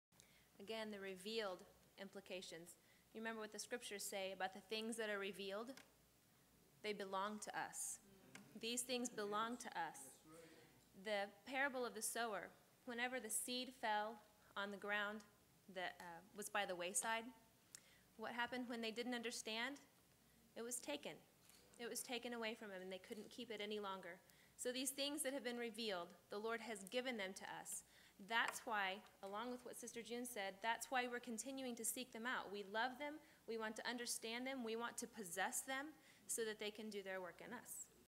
RWR20 - Response by Barb Hutchcraft - 2
Refreshing Waters Renewal #20 - The Revealed Implications of The Gospel -
Audience Discussion led by Aaron Hutchcraft